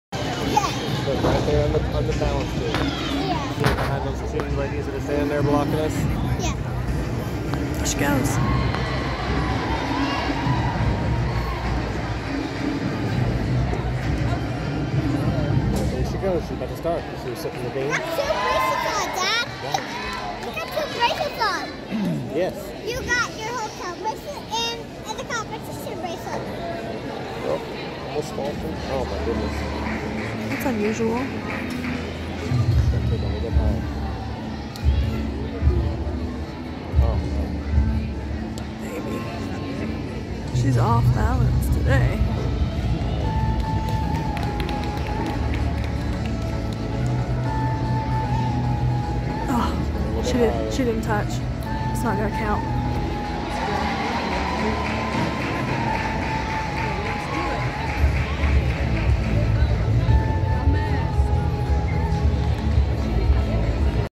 Maddie on beam at the Make It Count Spectacular!
This one is kind of hard to see, but here's Maddie on beam at the Make It Count Spectacular! StarsEverywhere kids gymnastics River City Turners Gymnastics